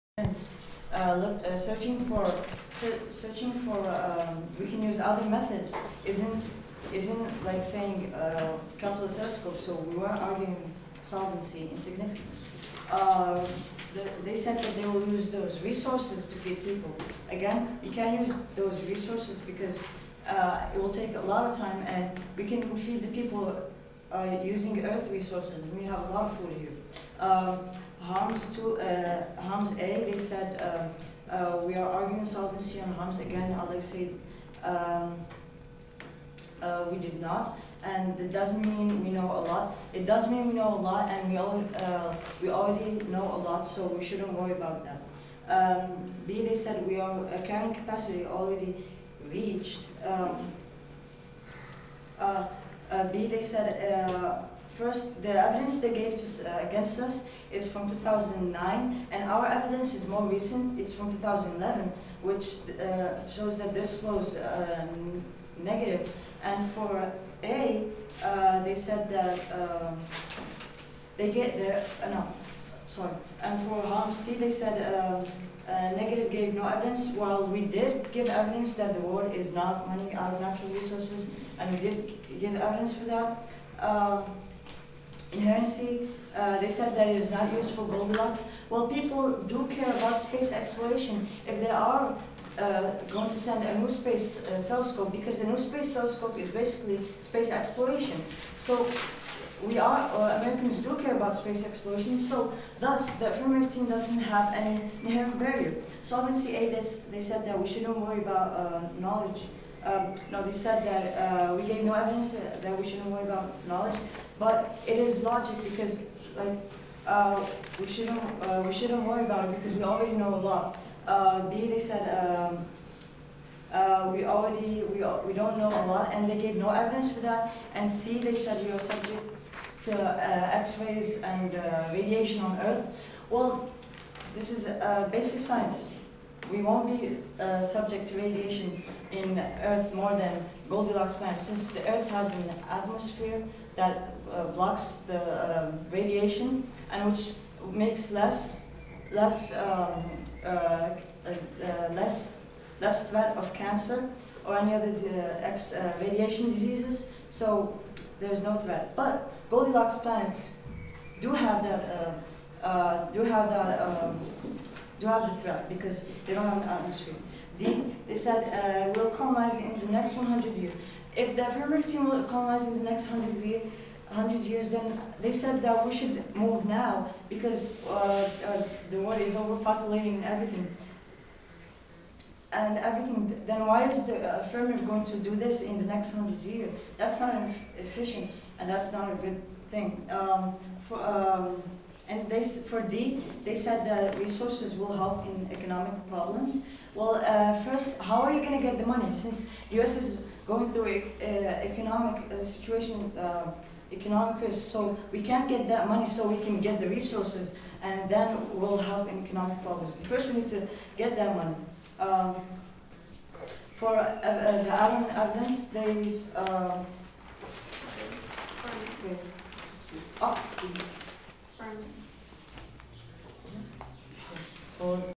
Morocco's 1st NFL Invitational Debate Tournament: 1NR
Discussion of Searching for Goldilocks Planets, 2nd Negative Rebuttal, at Amicitia Invitational Debate Tournament, Final Round.